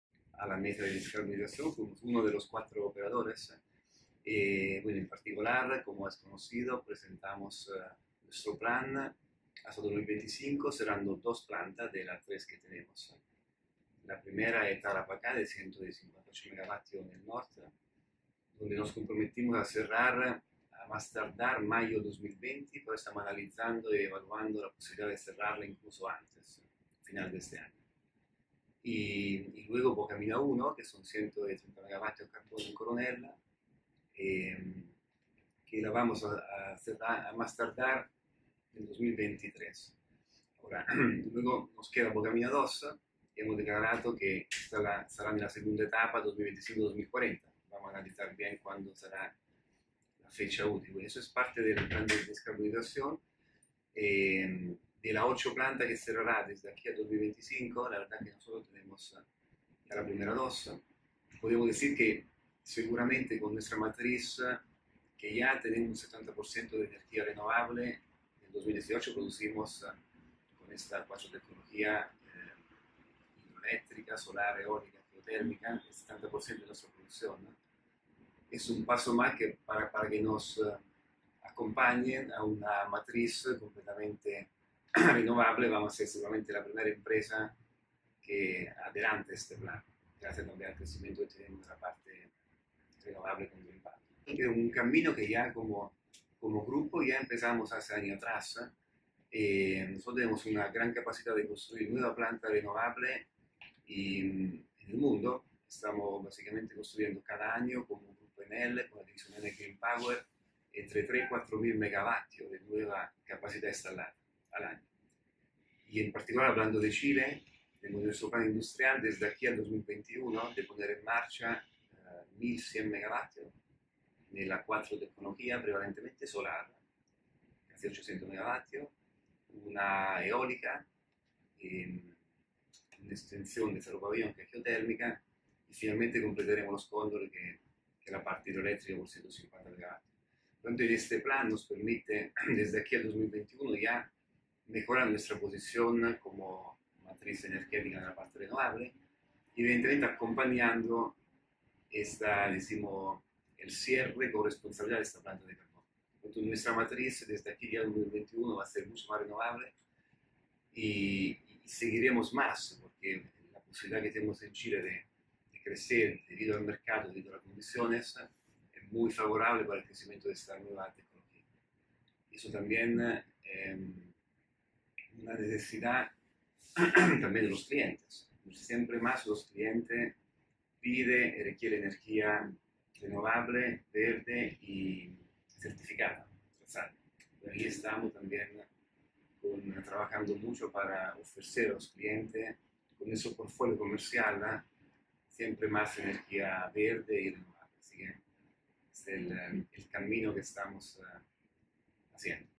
Ahora en DF: Enel detalla los avances del plan de descarbonización del grupo en el país
Como un plan “responsable” califica Valter Moro, gerente general de Enel Green Power Chile, el cronograma de descarbonización dado a conocer en Chile por el gobierno, con miras a 2040.